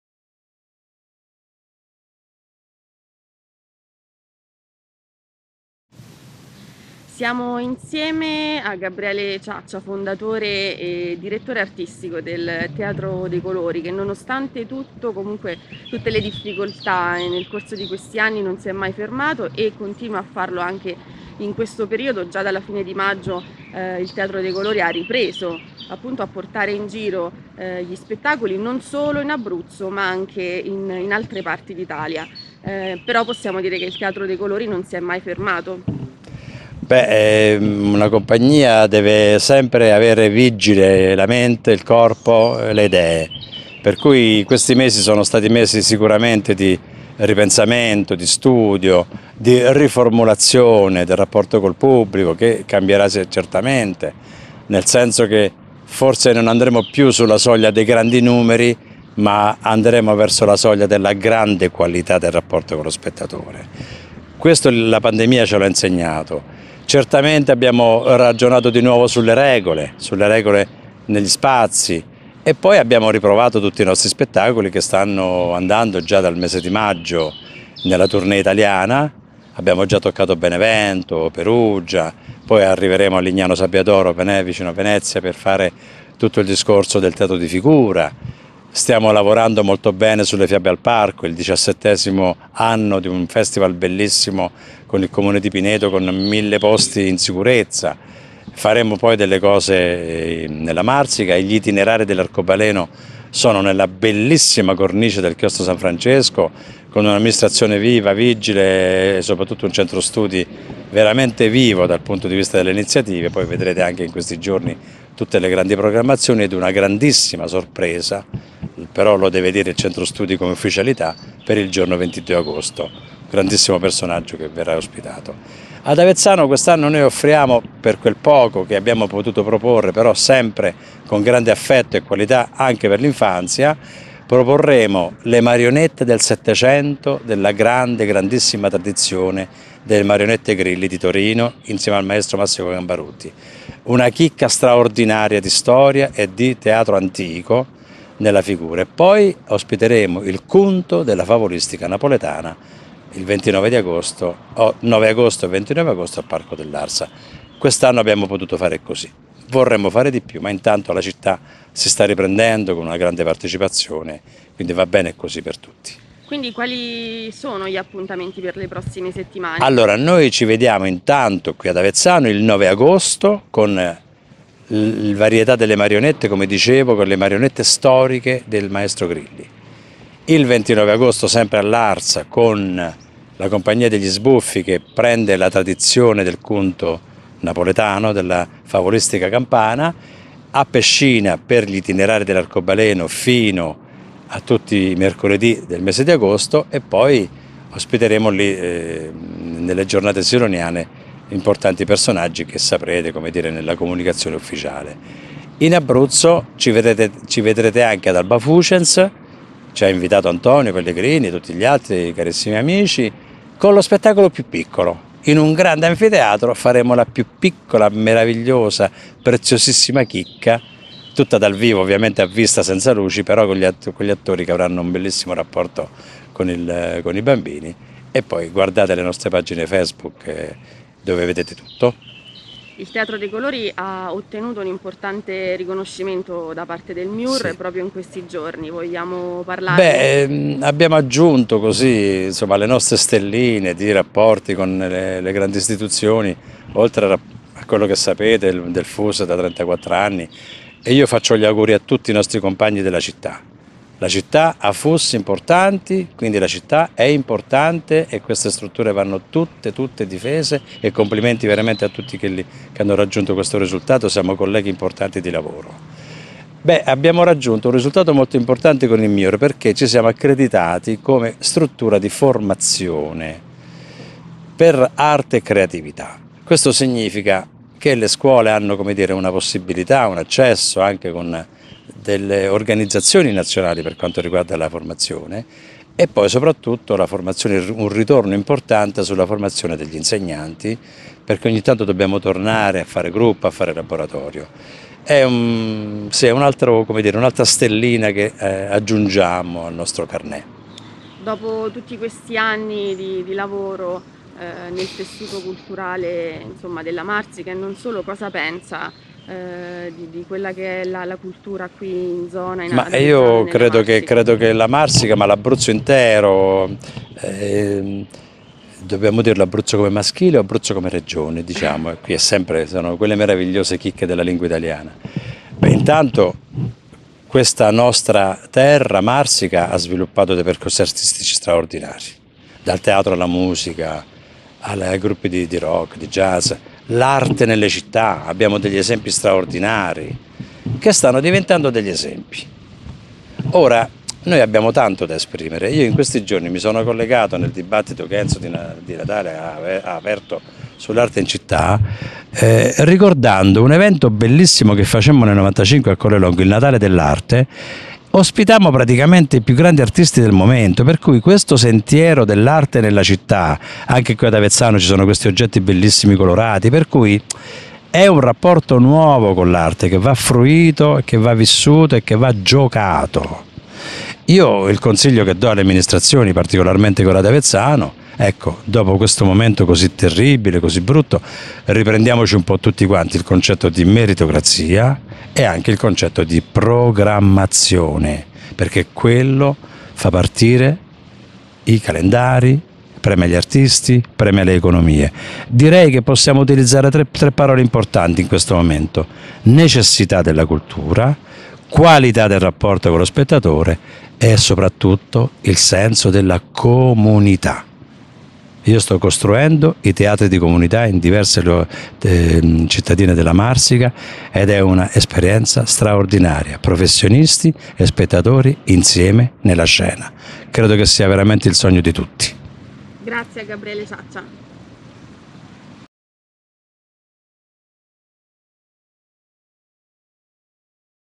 Intervista a Gabriele Ciaccia
Gabriele Ciaccia: intervista al fondatore e direttore artistico del Teatro dei Colori

A cura di Antonella Valente